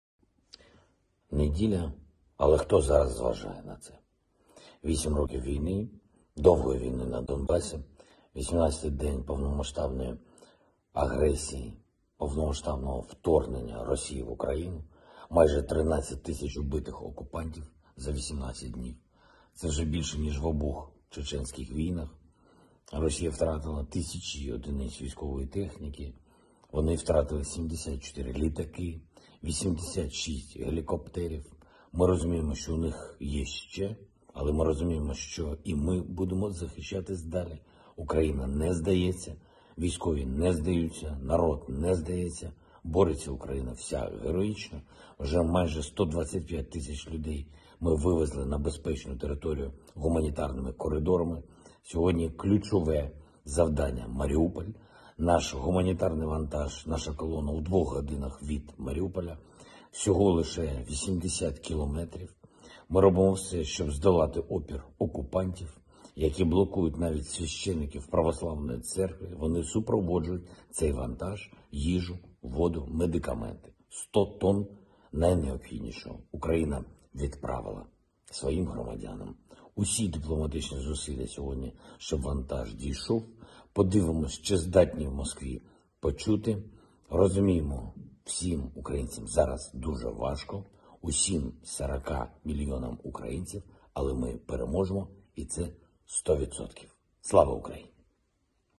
Zelensky Important about today About war We will win Ukraine does not give up,
Zelensky "Important about today. About war. We will win. Ukraine does not give up, the military does not give up, the people do not give up. Glory to Ukraine!"

More Muslim Chechen forces head towards Ukraine. Facing the ultra Nazi Christian militias.

Turning this war into a little Crusade of its own... Chechens are undertaking the most dangerous missions and that has greatly increased their respect within the Russian forces..

🌍Depositphotos Video Plans
Amazing collection of professional stock videos in 4K, HD and SD resolutions.